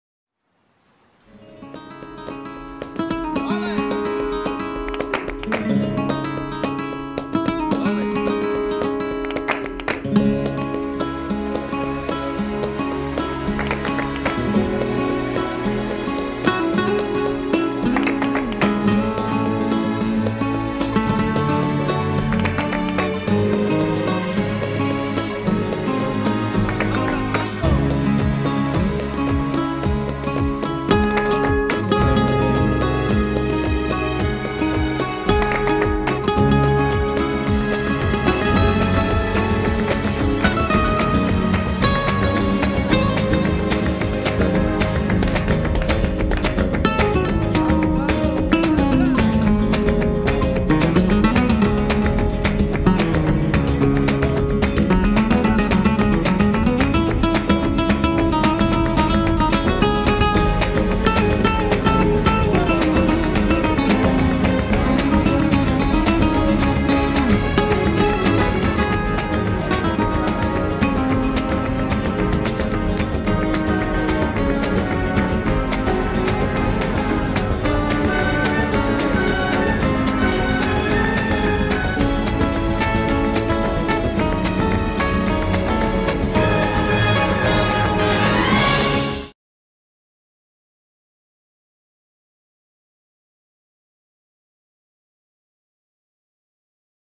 Flamenco dancer @laflamma in the Teatr na vode 's water show at crocuscitymall
О чем это видео: Flamenco dancer @laflamma in the Teatr na vode 's water show at crocuscitymall